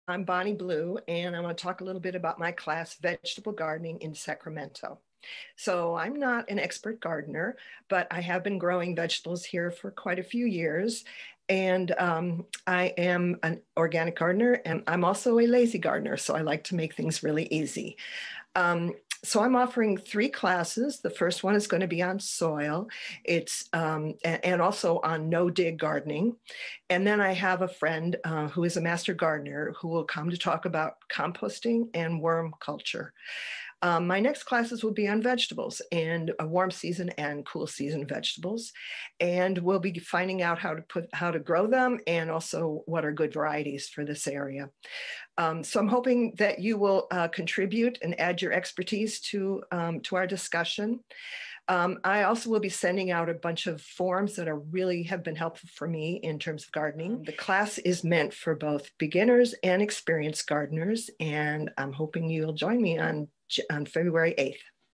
Vegetable Gardening in Sacramento with Bonnie Blue
Join Bonnie Blue in a three-part series about how to successfully garden in Sacramento.